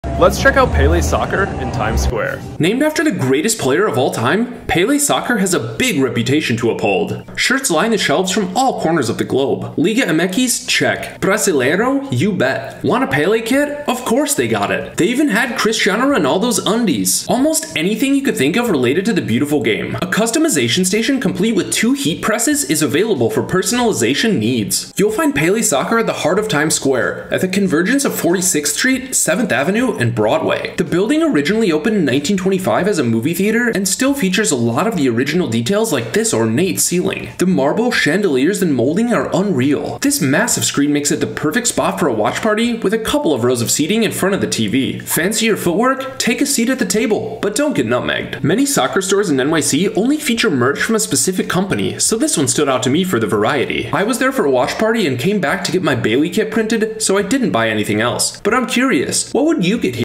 Phenomenal Pelé 🇧🇷🐐 | Pelé Soccer In NYC - NYC Soccer Stores
Have you been to Pelé Soccer in New York? This is one of the most comprehensive stores I’ve been to when it comes to football shirts, soccer jerseys and really anything related to the beautiful game!

With club and national team jerseys on display this was every soccer fan's dream. Would you give this place a visit?

What other soccer stores in NYC do you want to see?

📍 1560 Broadway, New York, NY 10036, United States

Pele Phenomenal Pelé 🇧🇷🐐 | Pelé Soccer In NYC - NYC Soccer Stores